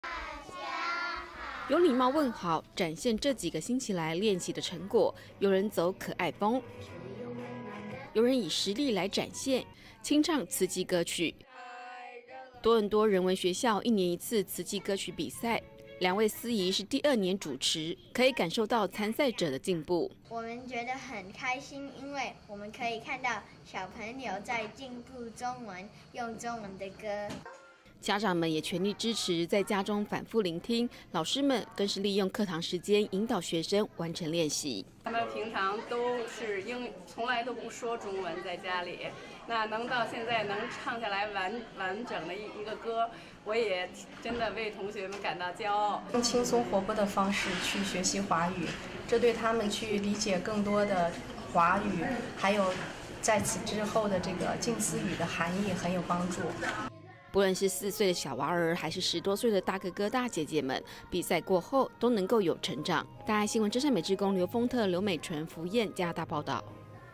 慈濟歌比賽 華語輕鬆學
慈濟加拿大多倫多人文學校，每年都會舉辦慈濟歌曲比賽，說說唱唱，學中文也學人文。

有禮貌問好，展現這幾個星期來，練習的成果，有人走可愛風。

有人以實力展現，清唱慈濟歌曲。

多倫多人文學校，一年一次，慈濟歌曲比賽，二位司儀，是第二年主持，可以感受到，參賽者的進步。

多倫多人文學校學生 蔡穎盈：「我們覺得很開心，因為可以看到小朋友在進步中文，用中文的歌。」

家長們，也全力支持，在家中反覆聆聽，老師們，也利用課堂時間，引導學生，完成練習。

多倫多人文學校老師 蔚紅：「他們平常都不說中文 在家裡，能到現在唱下來完整的歌，我也真的為同學們感到驕傲。」

多倫多人文學校老師 符燕：「用輕鬆活潑的方式去學習華語，這對他們去理解更多的華語，還有在此之後的靜思語含意，很有幫助。」

不論是四歲的小娃兒，還是十多歲的大哥哥，比賽過後都有成長。

真善美志工 劉豐特 劉美淳 符燕 加拿大報導